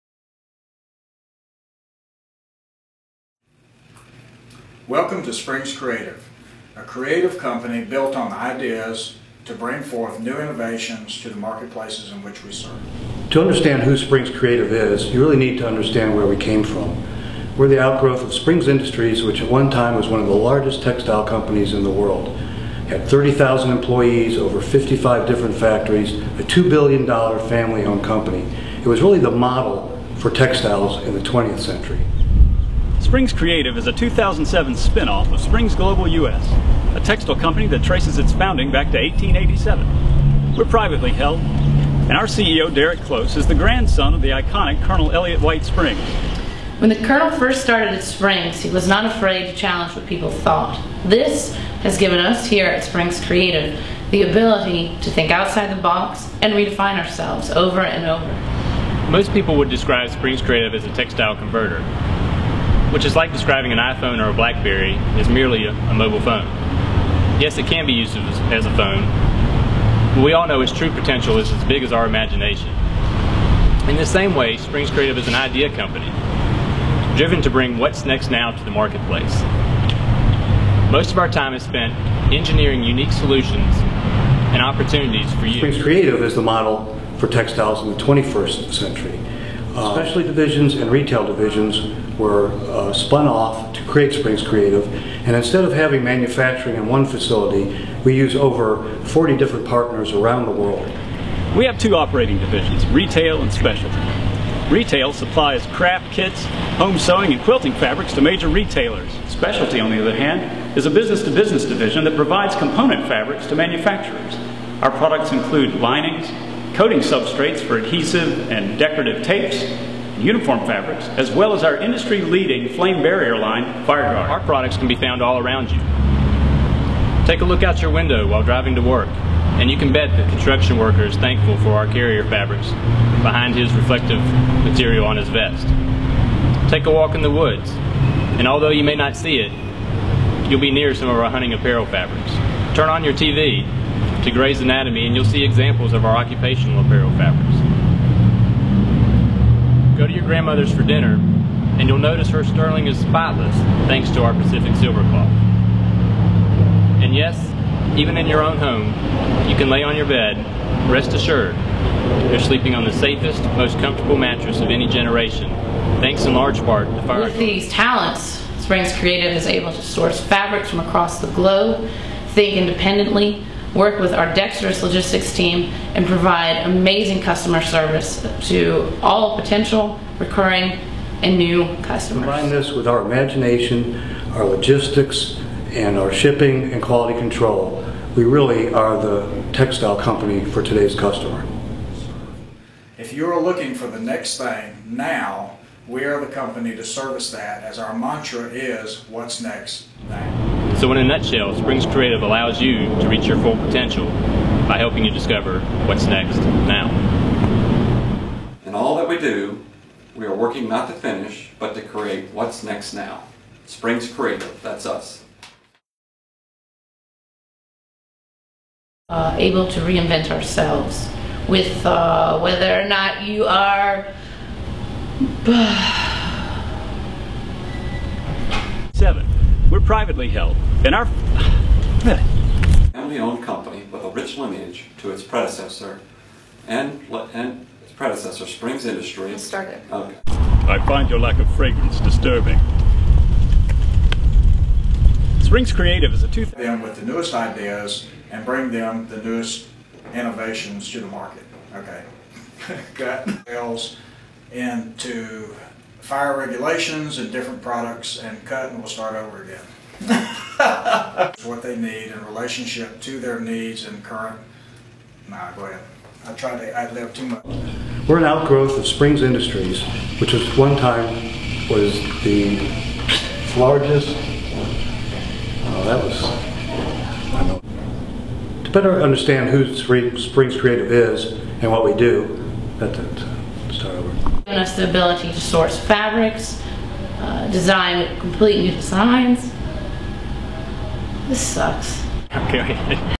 Springs Creative - Specialty Video -- What We Do2 - Cellular.m4v
A look into the world of Springs Creative Product Group's "Specialty Divison".